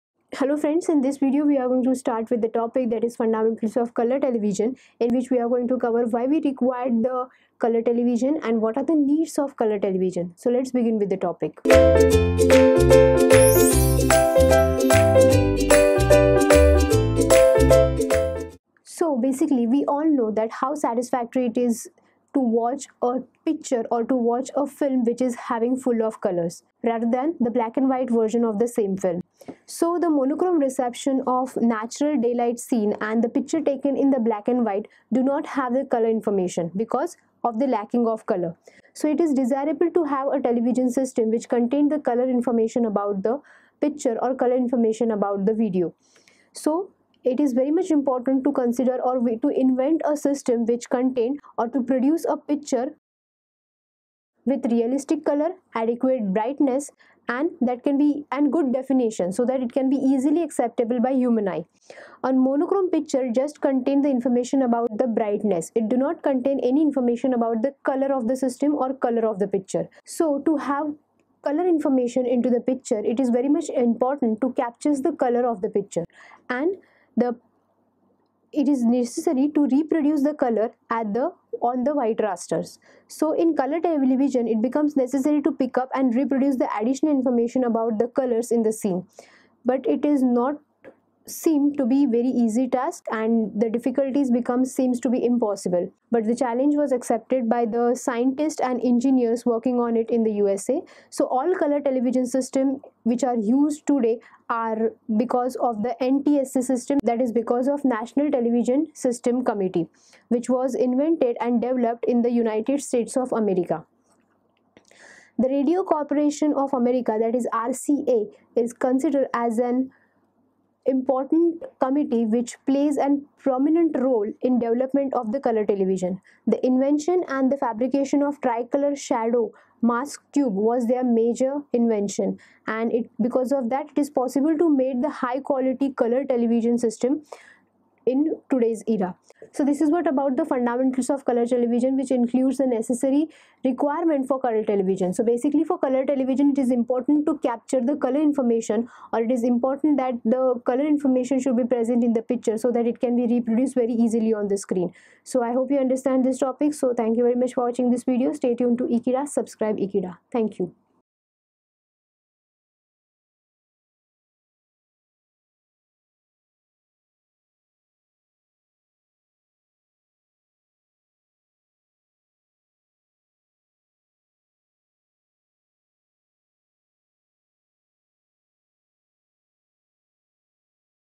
Fundamental of Colour Television | TV and Video Engineering
Explore the vibrant world of Color Television in this insightful video on TV and Video Engineering. Delve into the fundamentals, uncovering the core concepts that bring colors to life on your screens. From decoding the workings of color transmission to understanding the intricate mechanisms behind this technology, join us on a journey through the heart of television engineering. Whether you're a curious enthusiast or a budding professional, this session on the Fundamental of Color Television promises to unravel the mysteries behind the vivid images that grace your screens daily. Tune in and enrich your understanding of this fascinating field!

Happy Learning.